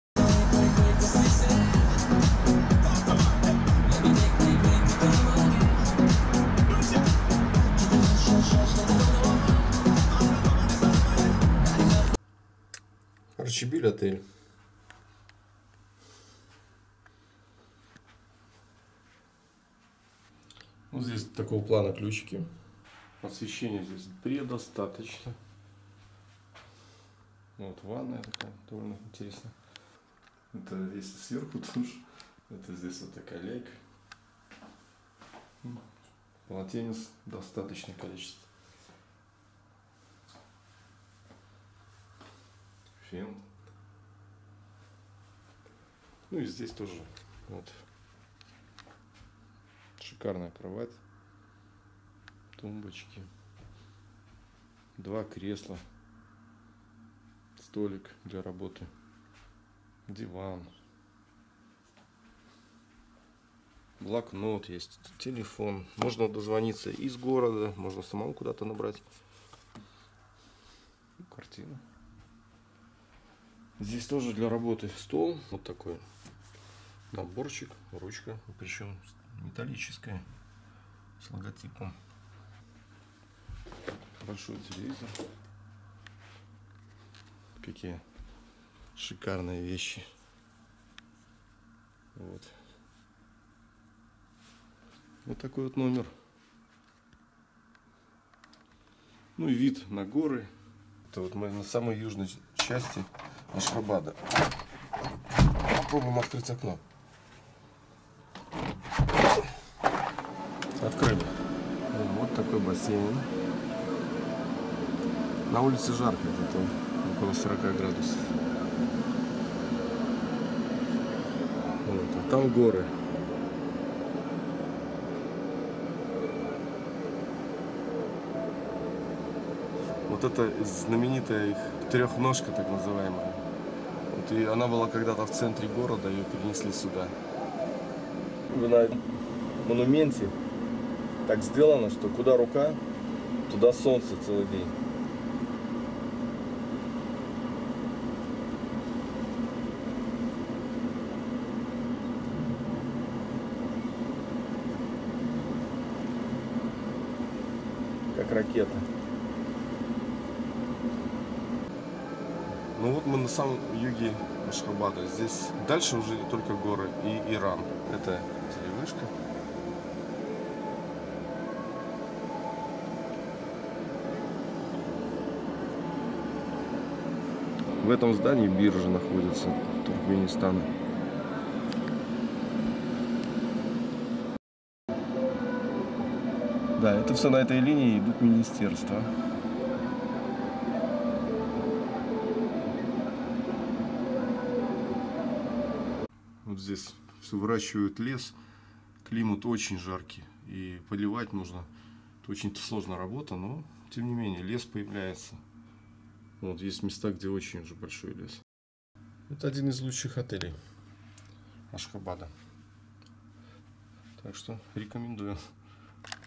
Туркменистан/Ашхабад/Отель Арчабиль
На юге Ашхабада располагается отель Арчабиль.
Это один из лучших отелей Туркменистана.
Если окна отеля выходят на южную сторону, то открывается живописный вид на Копетдагские горы, а также на монумент "Арка Нейтралитета", в народе именуемую "Трёхножкой".
С северной стороны окна выходят на город Ашхабад.